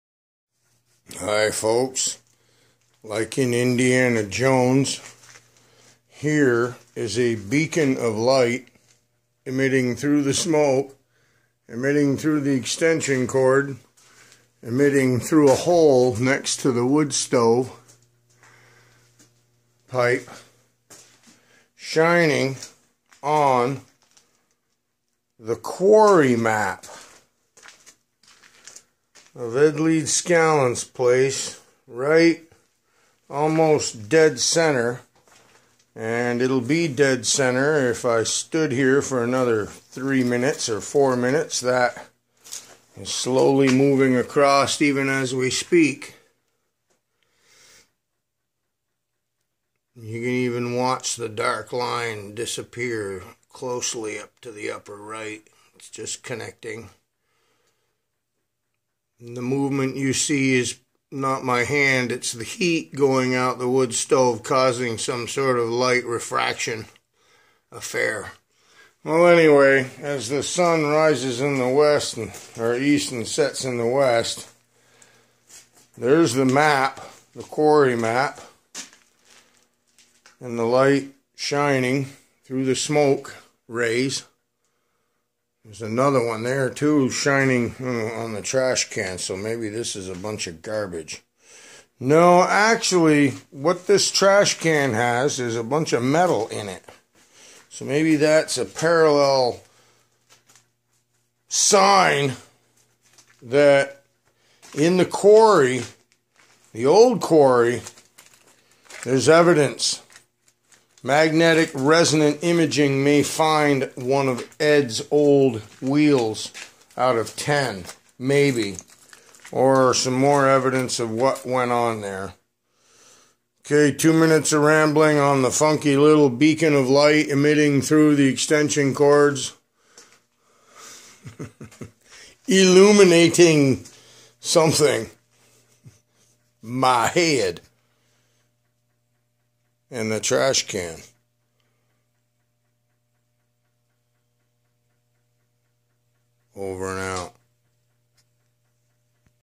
Indiana Jones? Buried treasure at Coral Castle? Scott and Roy video?
REALLY FAR OUT THERE VIDEO. open your mind. You never know. Coral Castle might have an alignment where the OBELISK / SIGHT GLASS (N + S meridian) and the SUNDIAL (E +W)  for an X MARKS THE SPOT.   just a thought.